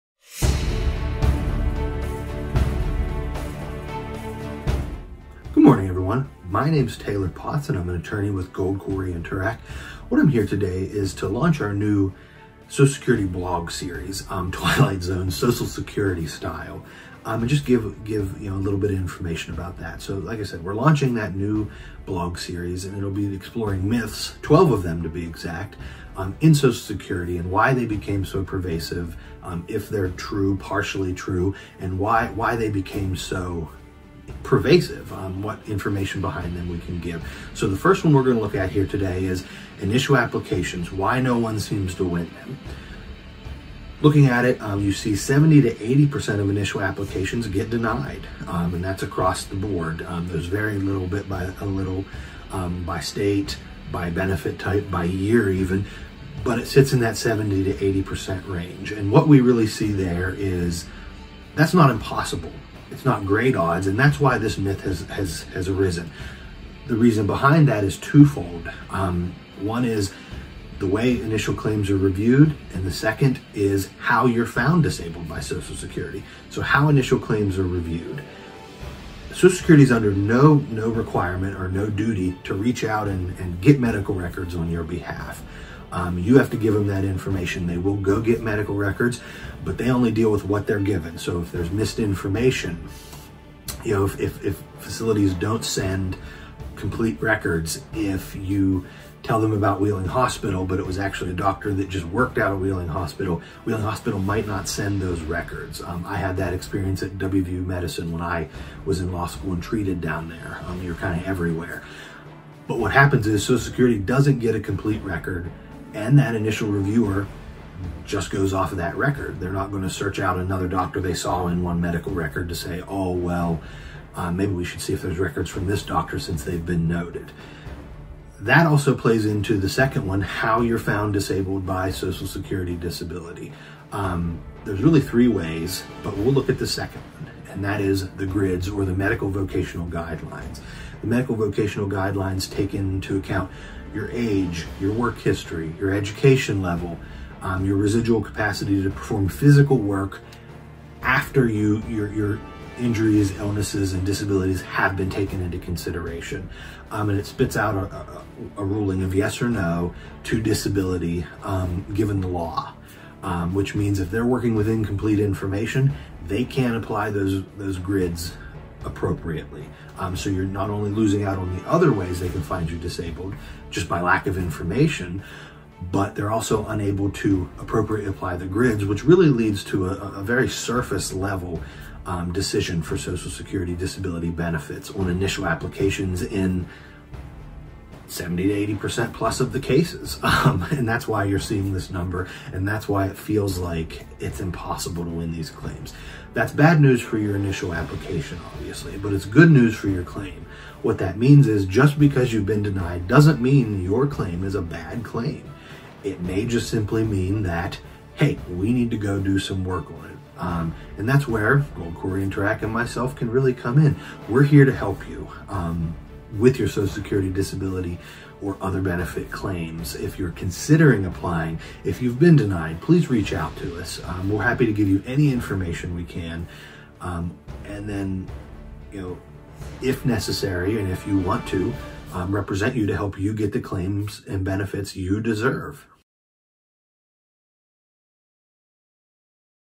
GKT Taylor Social Security Myths 1
GKT's Taylor Potts busts some of the myths going around about Social Security.